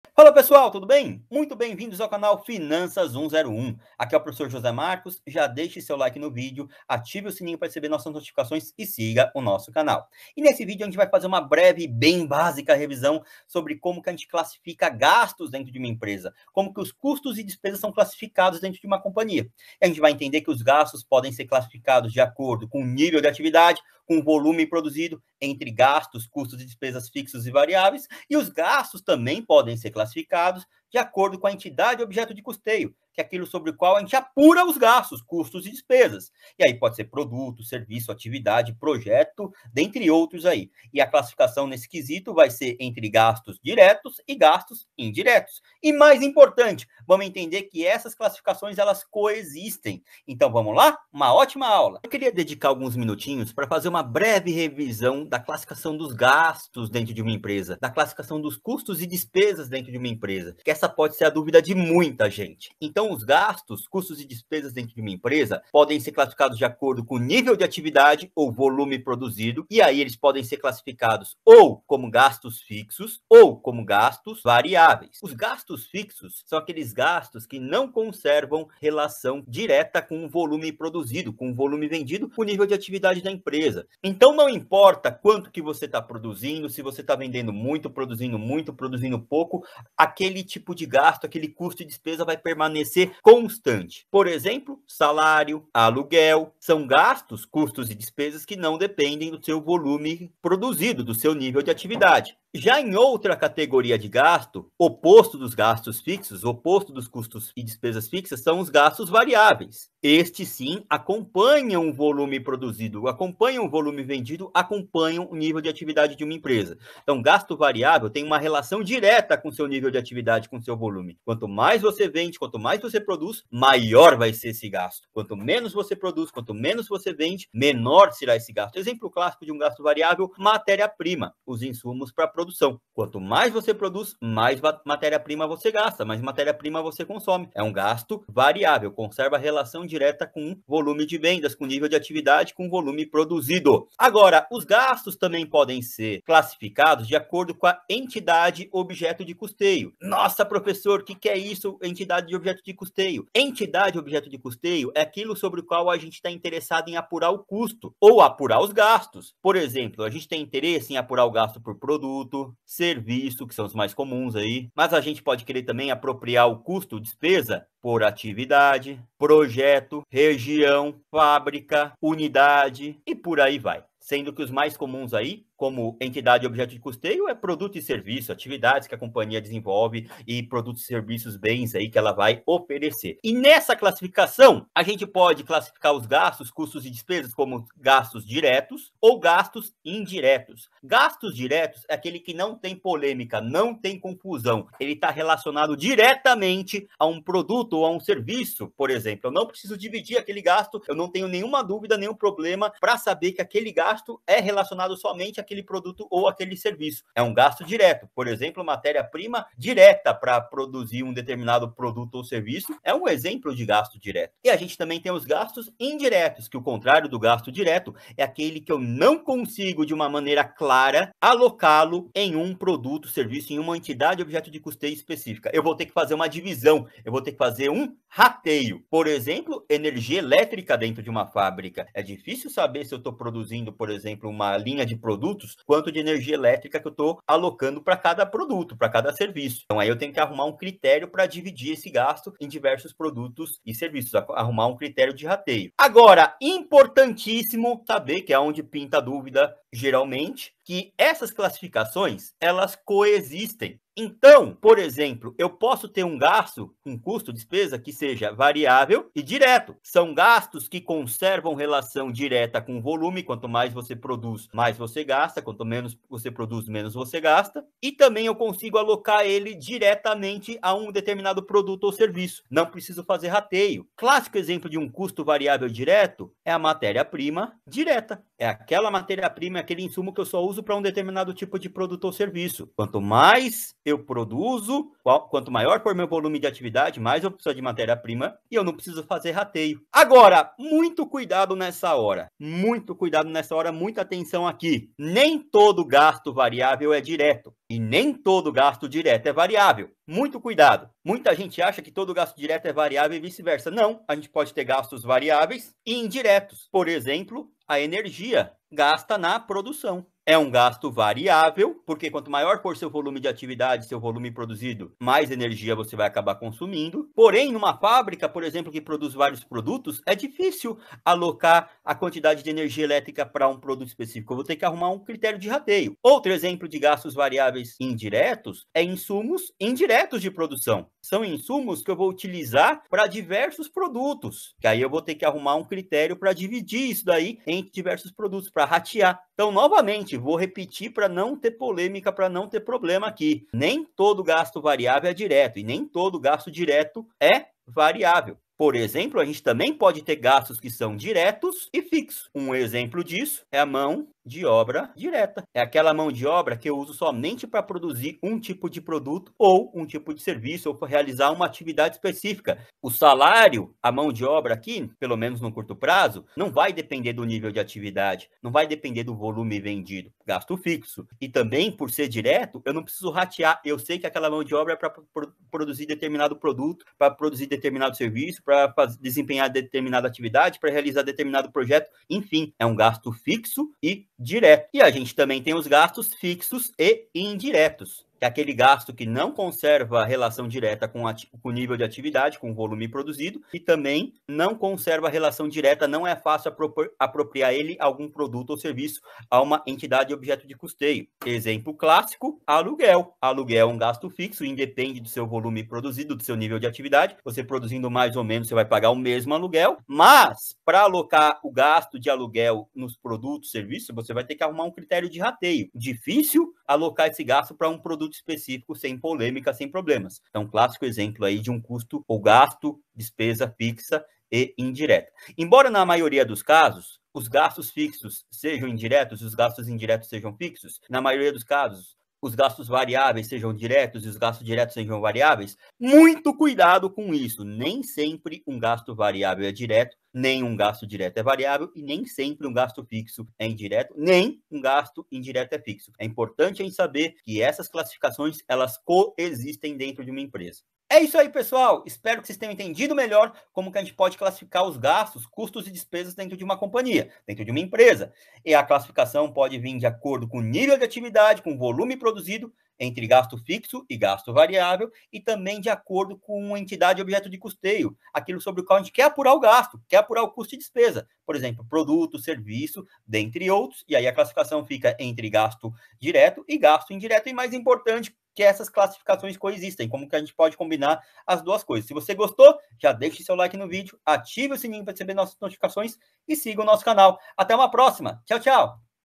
Classificação dos Gastos: Custos e Despesas
Entenda a classificação dos gastos, custos e despesas de uma empresa de acordo com o nível de atividade (volume de produção) e à entidade objeto de custeio (produto, por exemplo).

Em relação ao volume de produção os gastos podem ser classificados em fixos e variáveis. E em relação à entidade objeto de custeio os gastos podem ser classificados como diretos e indiretos.

Mestre em Finanças pela USP e Doutor em Finanças pela FGV